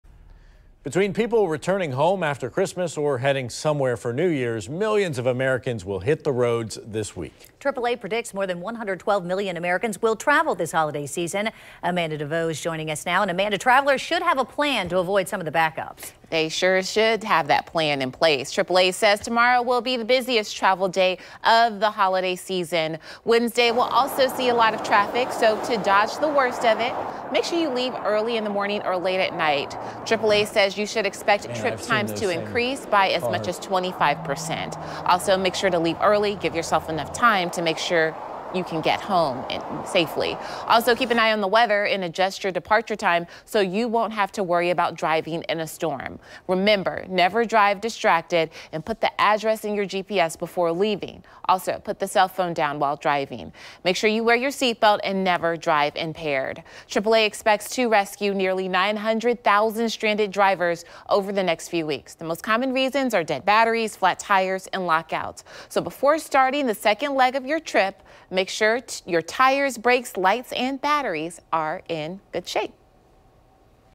Holiday travel: The best, worst time to hit the road
’Tis the season to travel, and AAA estimates 112.7 million people will journey 50 miles or more away from home from December 23 to January 2.